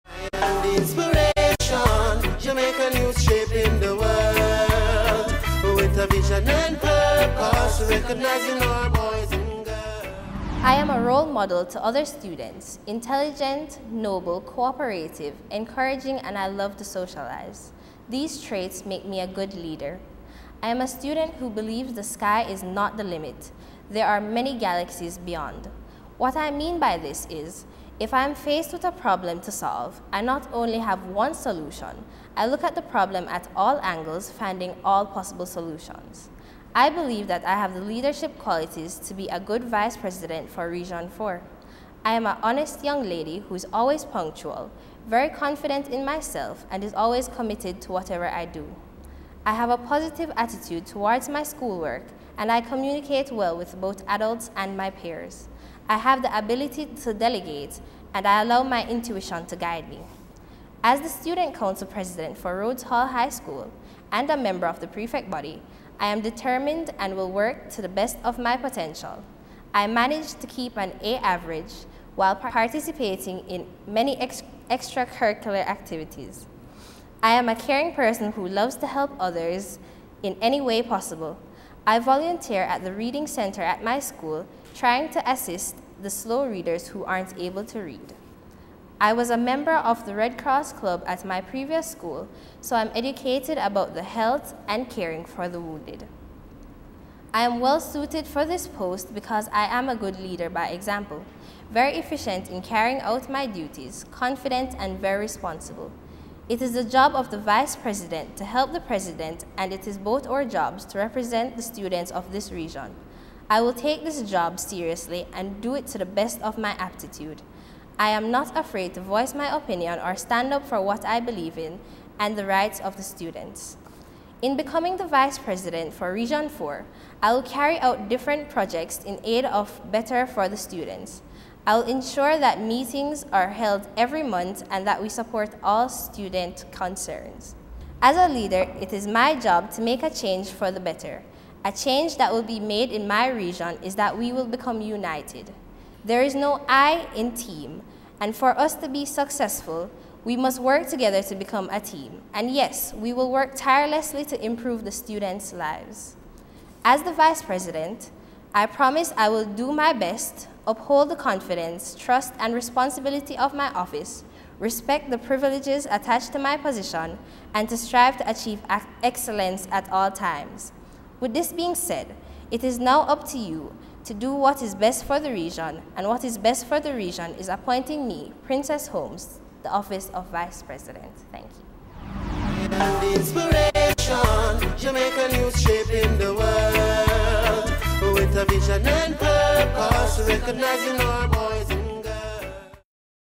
Princess Holmes (Rhodes Hall High): NSSC Elections 2013
The National Secondary Students' Council (NSSC) is the governing body for students' councils in Jamaica. The NSSC promotes students rights and responsibilities, youth advocacy, and the active participation of students in matters of civic and national interest. The Council remains guided by the principle that "Responsible students make the difference"

Election of the NSSC Executive Body for the 2013/14 academic year will be hosted on November 14 and 15.